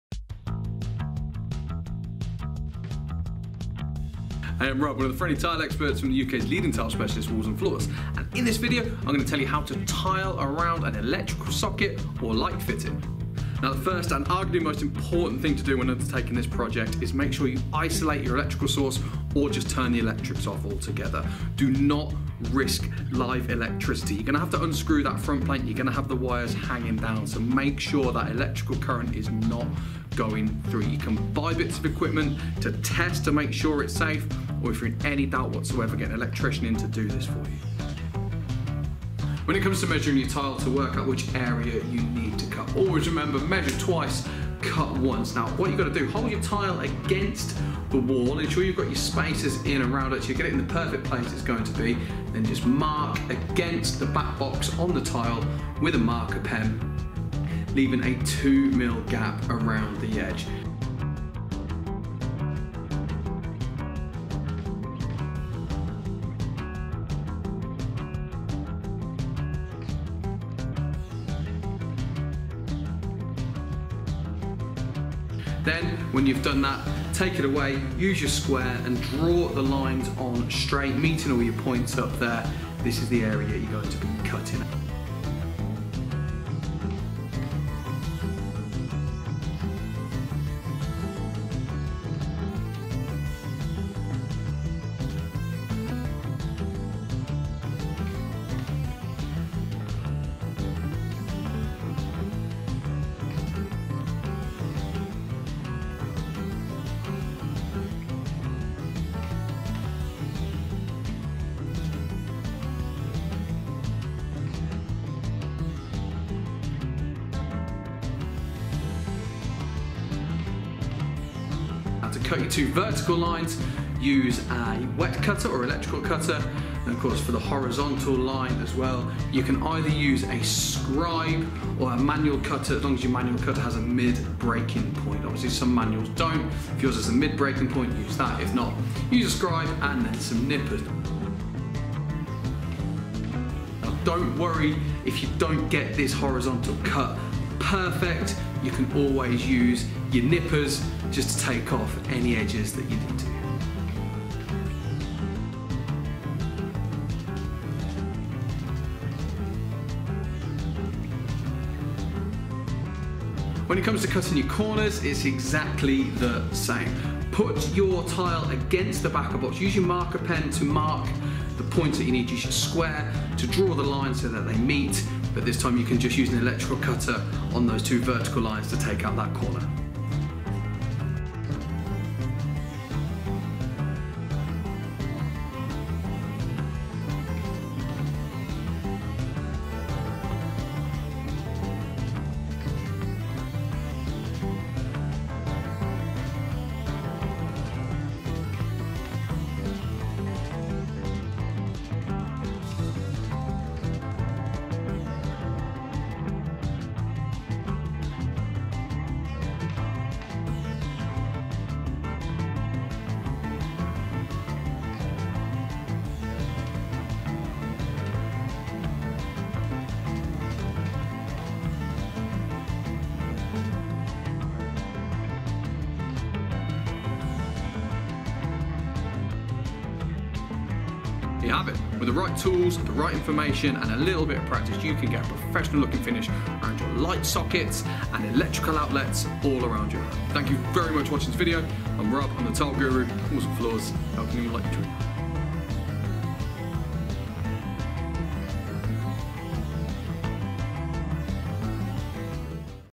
How to tile around an electrical socket
Plugs and electrical sockets are an essential part of modern life, so when it comes to unlocking your dream home its highly likely that you will come across a socket or light switch.

If you have ever wondered how you can tile around these obstacles  and still maintain a professional look?

This handy video shows you just how to tile around any socket and end up with a flush finish you can be proud of